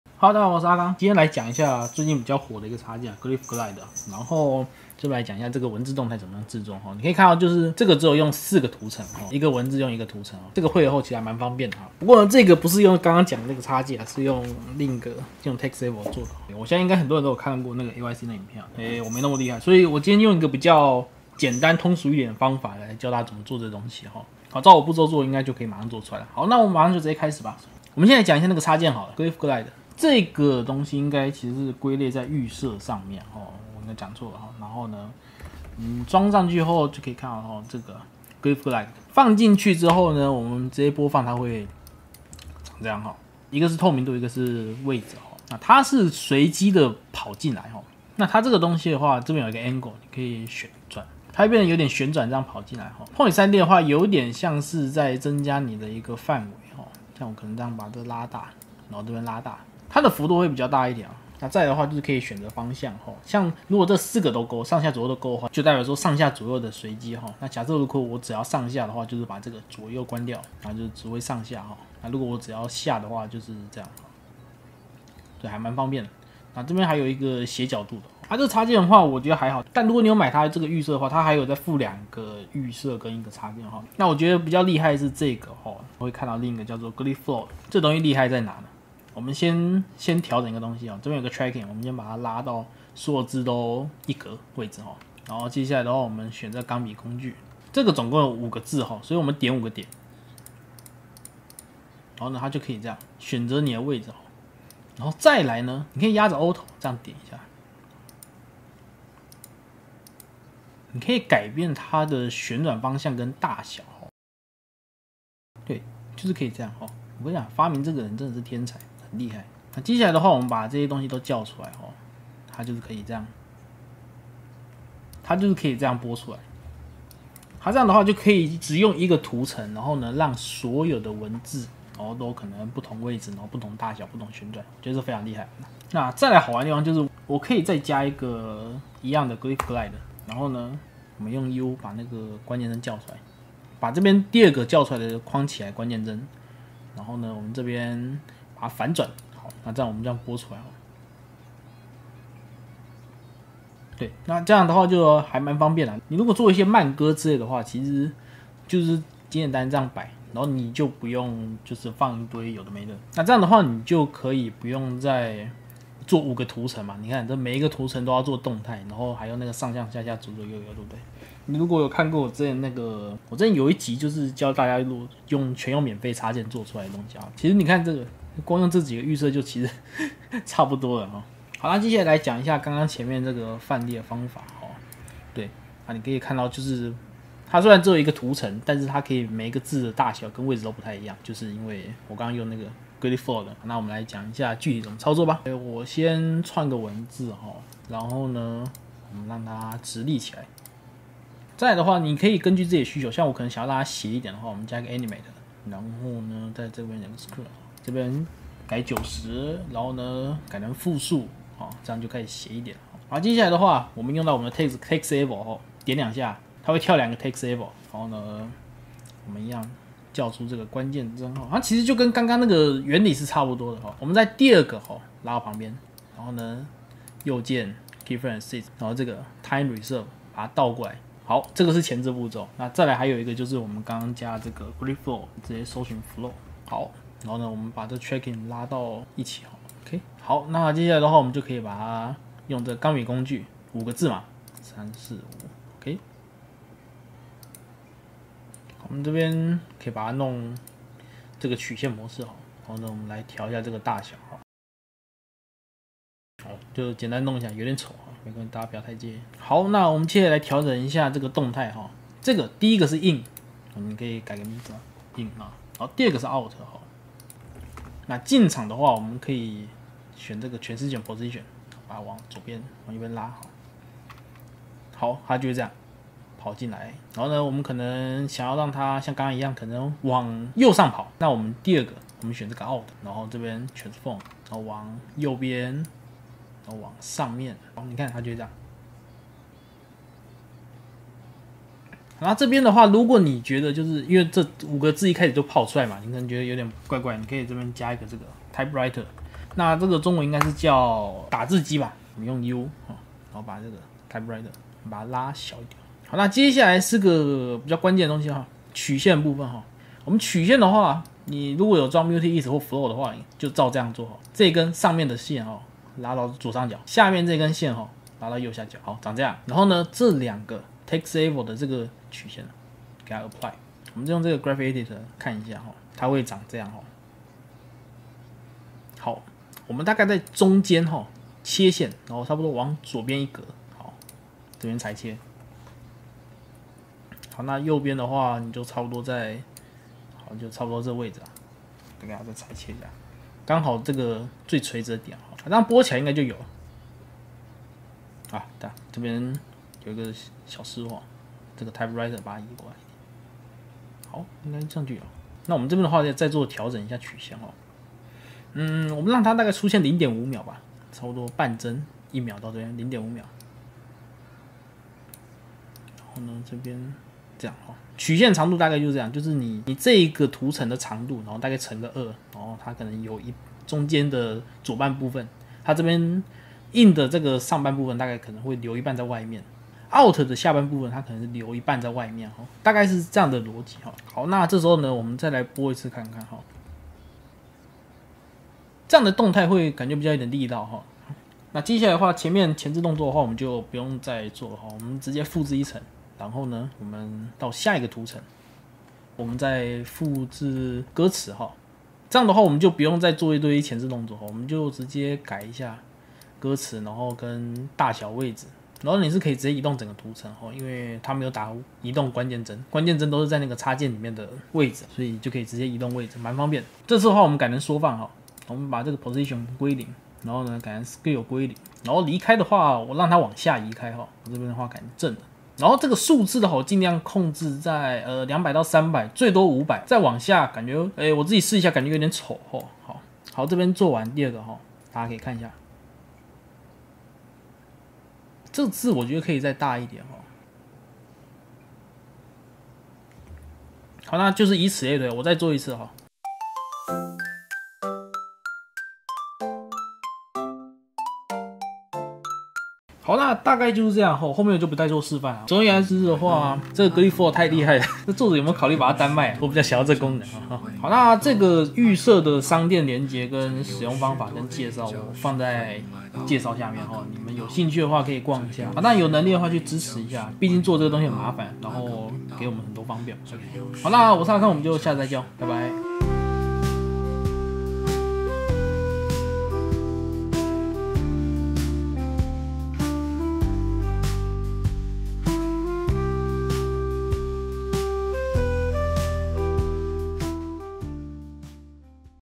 After Effect PV文字教學#18 glyph glide 、textevo文字
00:00 前言
00:27 介紹glyph glide
01:19 介紹glyph flow
03:01 文字製作方法(textevo+glyph flow)

➡AAAA!!我ら國動海賊團☆(注意音量)(EARRAPE WARNING)

➡小遊戲-國動在哪裡?

➡哈阿阿！！

➡實況主變成手遊廣告角色 LORD OF STREAMER